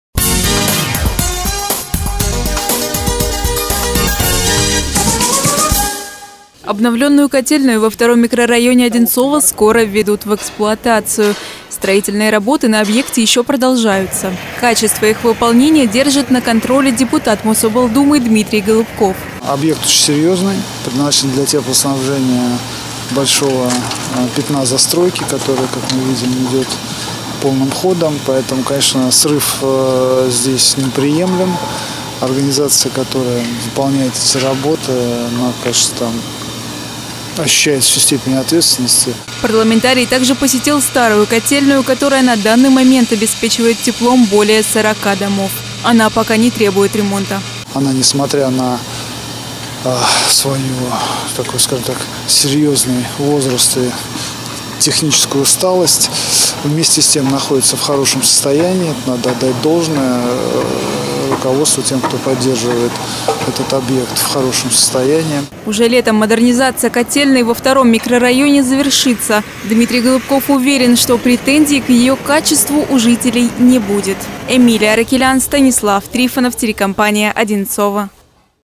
Новая котельная
Ход модернизации котельной во втором микрорайоне Одинцово проверил депутат Мособлдумы Дмитрий Голубков.